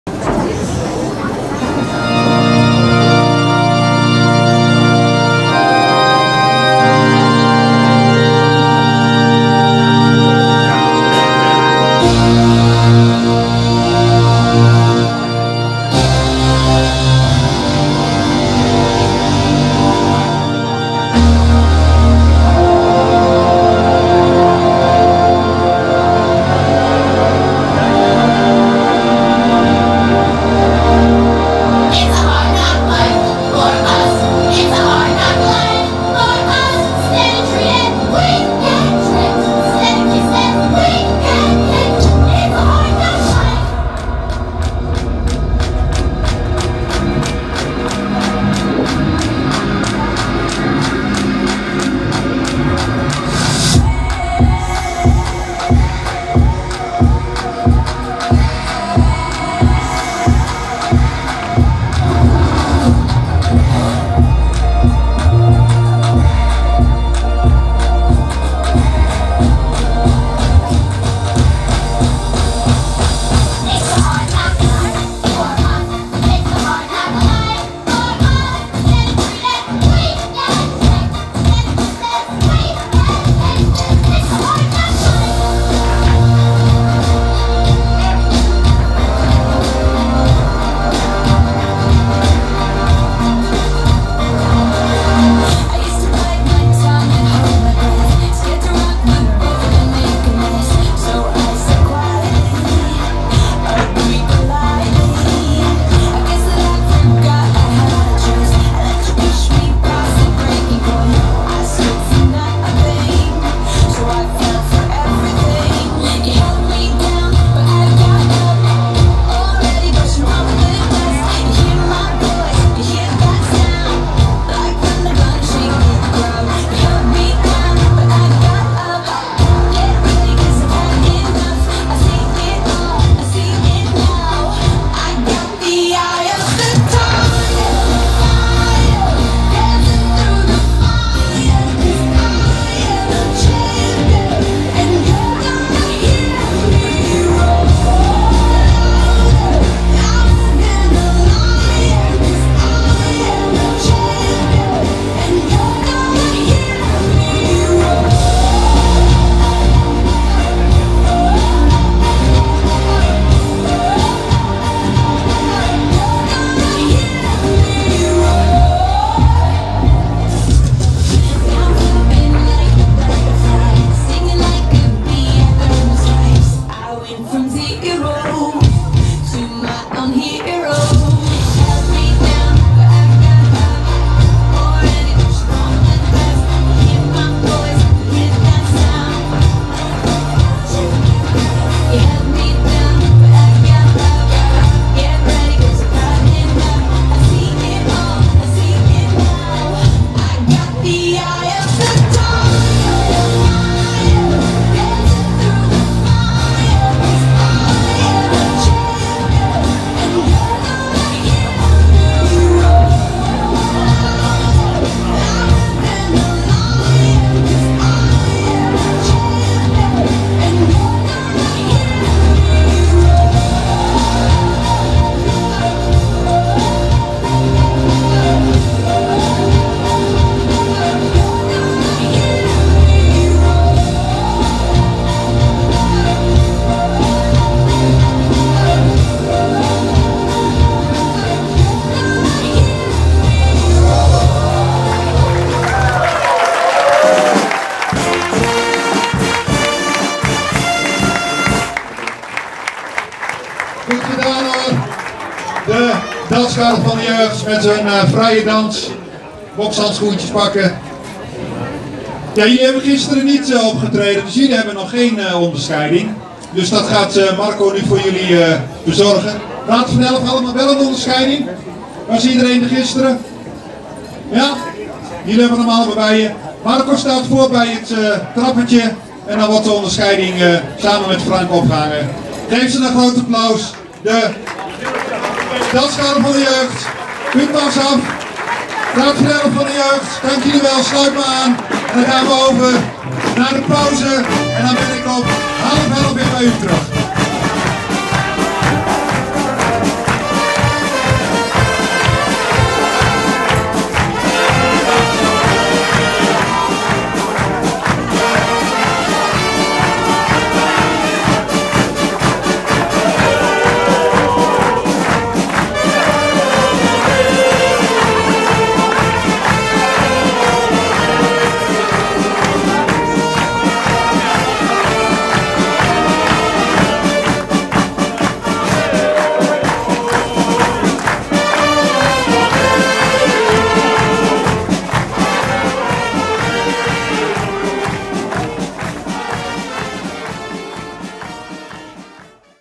Jeugdcarnaval Silvolde - Vrije dans
Er werd wat gedanst op het podium afgelopen weekend. Ook danst er elke avond een groep van Stichting Jeugdcarnaval Silvolde op ons podium. Op zaterdag was het de beurt aan de meiden van groep 7 die deze stoere dans lieten zien! Ook zij redden zich d'r wel met!